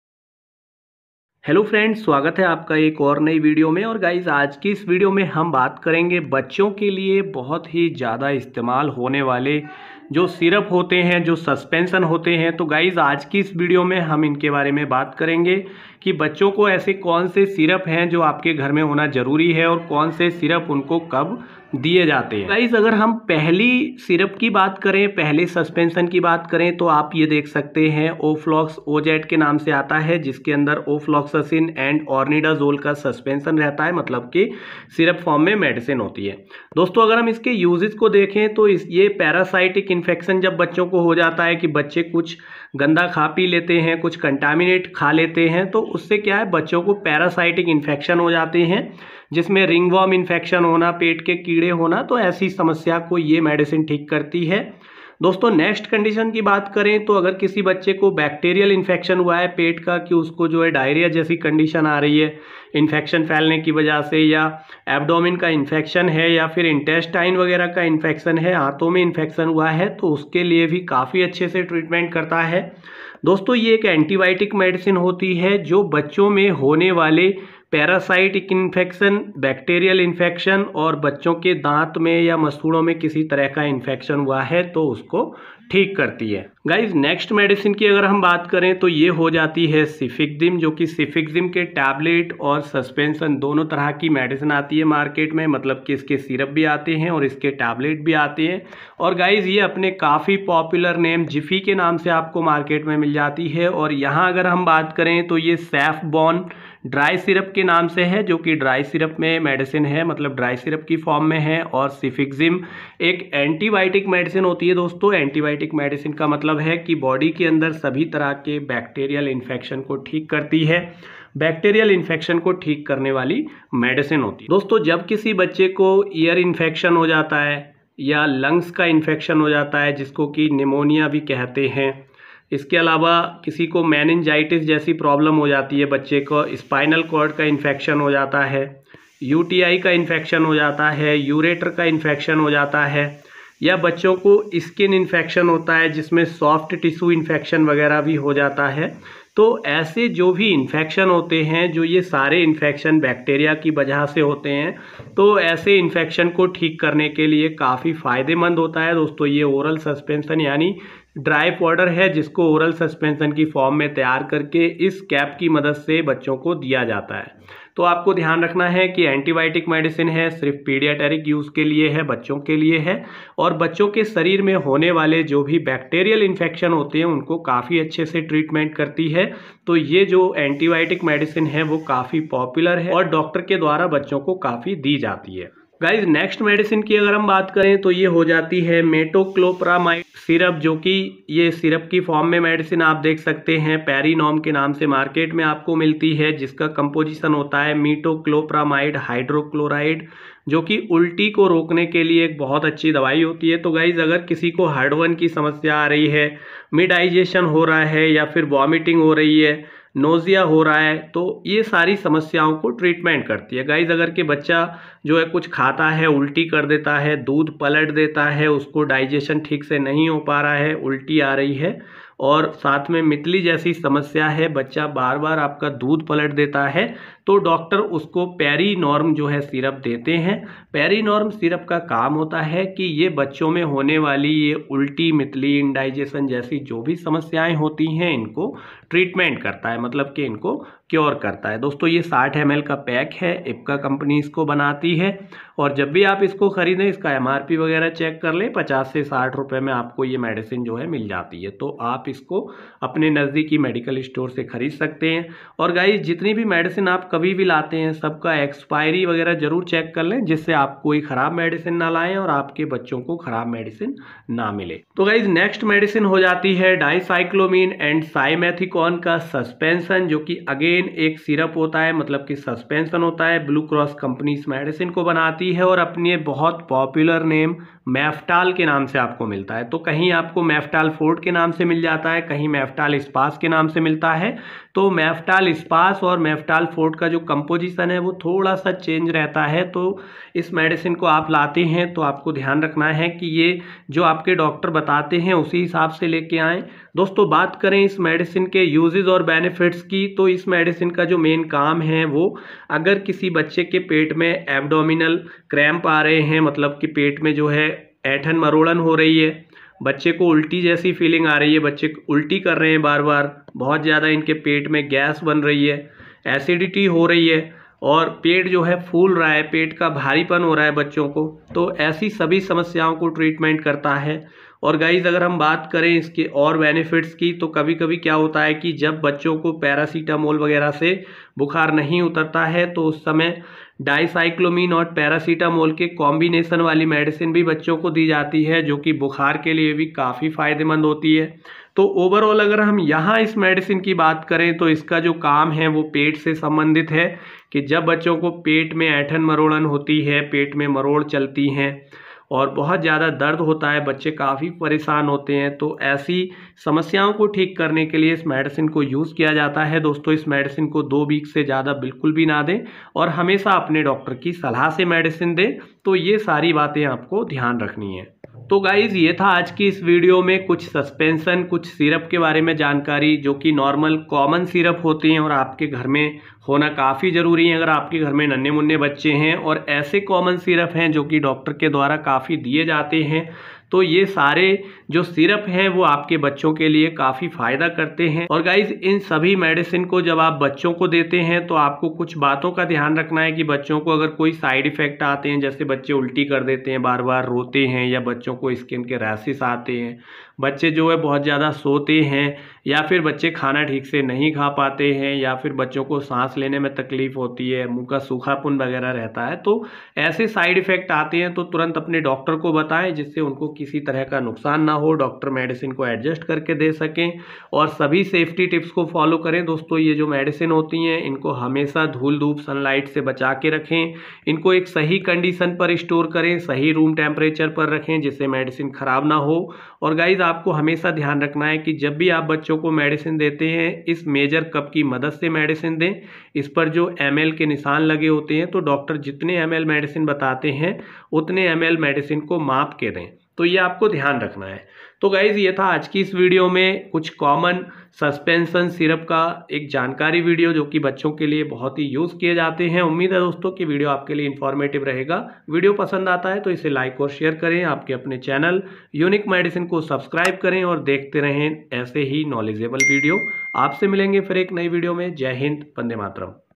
छोटे बच्चों मे ज्यादतर प्रयोग होने वाले Syrup !! Common Syrup & Suspension for Children
छोटे बच्चों मे ज्यादतर प्रयोग होने वाले Syrup |Commonly Used Syrup in Pediatric | Unique Medicine

Common Syrup & Suspension for Children You must keep at Your Home !! बच्चों के लिए Common दवाईयां